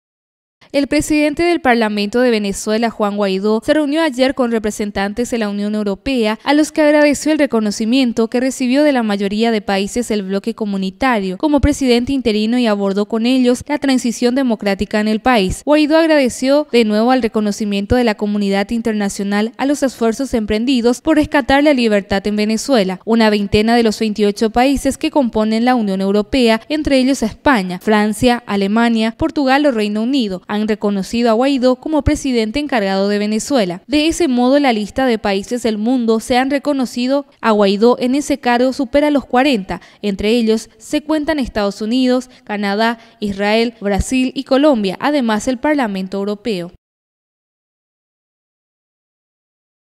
Guaidò con miembros de UE- Tres Fronteras Televisión
El presidente del Parlamento de Venezuela, Juan Guaidó, se reunió ayer con representantes de la Unión Europea (UE) a los que agradeció el reconocimiento que recibió de la mayoría de países del bloque comunitario como presidente interino y abordó con ellos la transición democrática en el país.

Guaidó agradeció de nuevo el reconocimiento de la comunidad internacional a los esfuerzos emprendidos por rescatar la libertad en Venezuela. Una veintena de los 28 países que componen la UE, entre ellos España, Francia, Alemania, Portugal o Reino Unido, han reconocido a Guaidó como presidente encargado de Venezuela.

De ese modo, la lista de países del mundo que han reconocido a Guaidó en ese cargo supera los 40. Entre ellos se cuentan EEUU, Canadá, Israel, Brasil y Colombia, además del Parlamento Europeo.